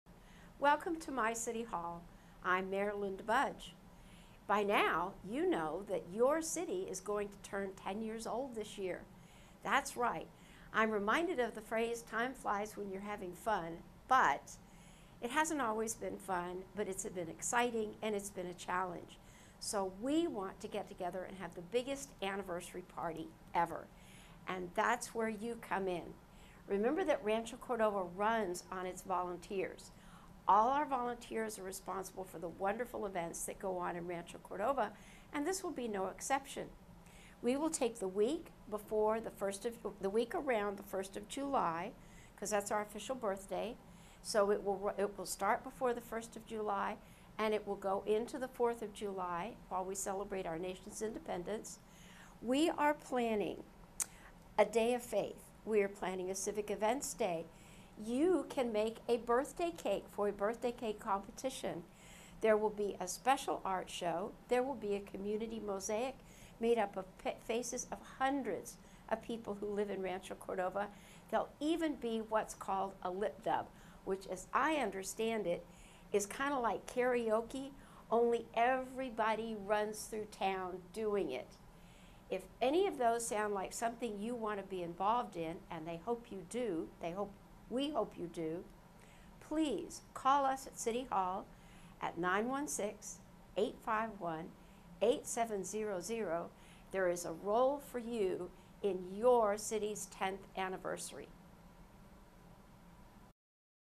Live from City Hall - Rancho Cordova Turns 10
Rancho Cordova Mayor Linda Budge has launched a video series "Live from City Hall!" Visit the City's YouTube page each week for updates and to view recent coverage.